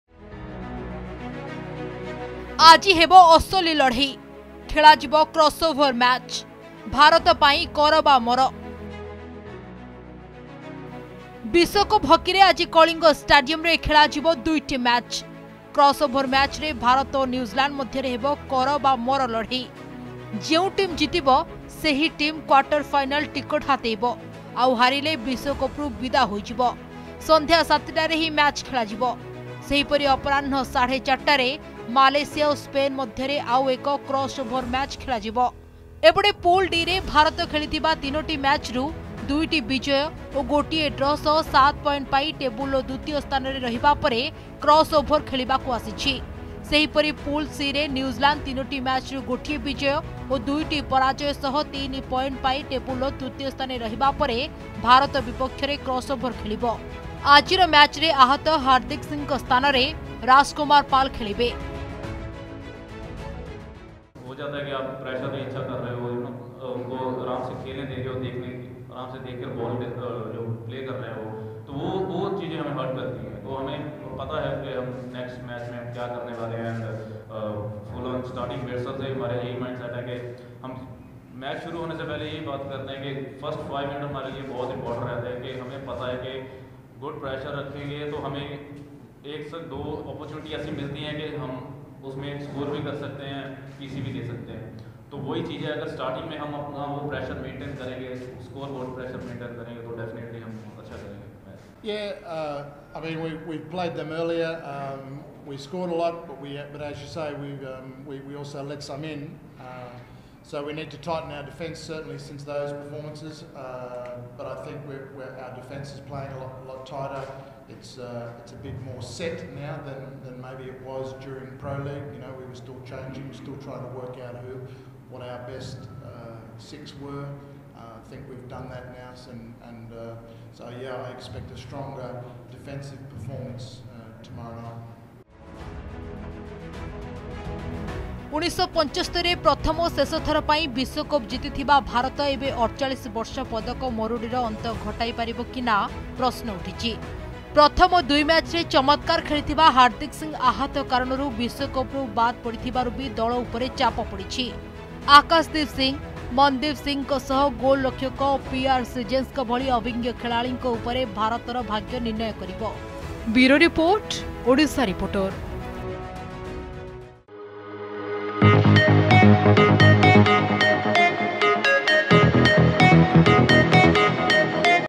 ହାର୍ଦ୍ଦିକ ବଦଳରେ ଖେଳିବେ ରାଜ କୁମାର | Odisha Reporter
ବିଶ୍ୱକପ୍‌ ହକିରେ ଆଜି କଳିଙ୍ଗ ଷ୍ଟାଡିୟମ୍‌ରେ ଖେଳାଯିବ ଦୁଇଟି ମ୍ୟାଚ୍‌ । କ୍ରସ୍‌ ଓଭର ମ୍ୟାଚ୍‌ରେ ଭାରତ ଓ ନ୍ୟୁଜିଲାଣ୍ଡ ମଧ୍ୟରେ ହେବ କର ବା ମର ଲଢେଇ । ଯେଉଁ ଟିମ୍‌ ଜିତିବ, ସେହି ଟିମ୍‌ କ୍ୱାର୍ଟର ଫାଇନାଲ ଟିକଟ ହାତେଇବ। ଆଉ ହାରିଲେ ବିଶ୍ୱକପ‌ରୁ ବିଦା ହୋଇଯିବ। ସନ୍ଧ୍ୟା ୭ଟାରେ ଏହି ମ୍ୟାଚ ଖେଳାଯିବ।